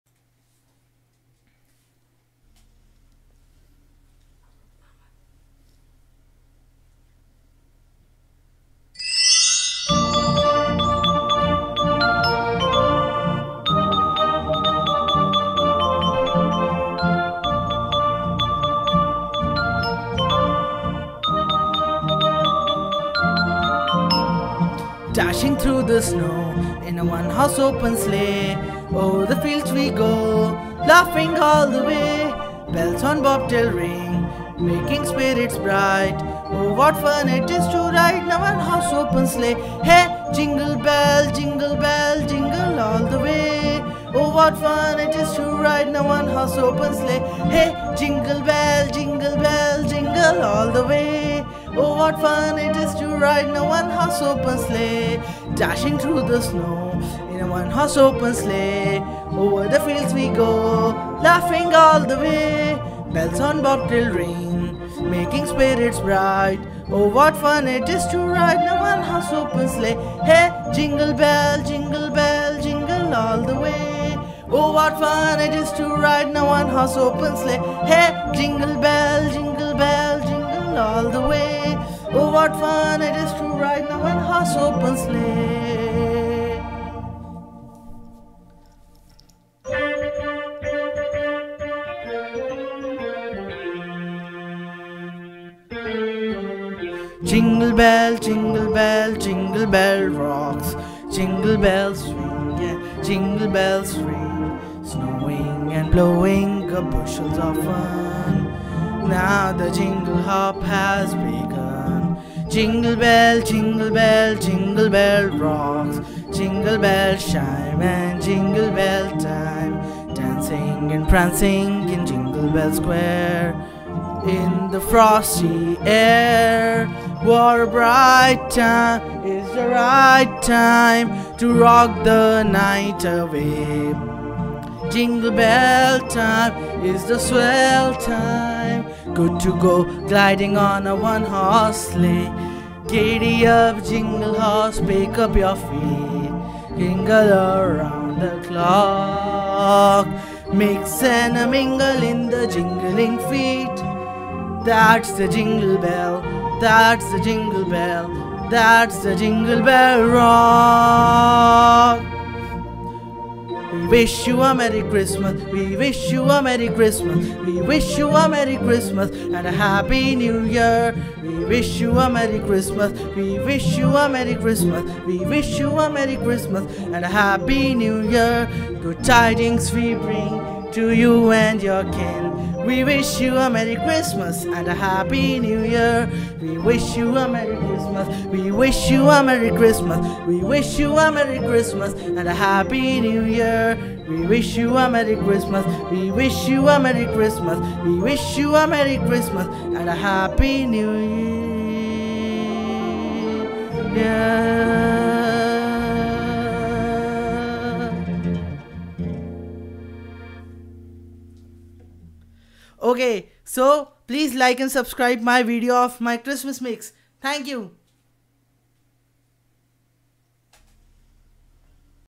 Christmas Delight | Ruchir | Christmas Celebration Mix | 3 Songs For Christmas
Its The Time Of The Year When We Should Avoid Our Diffrences And Celebrate Together.
I Hope That You Enjoyed It.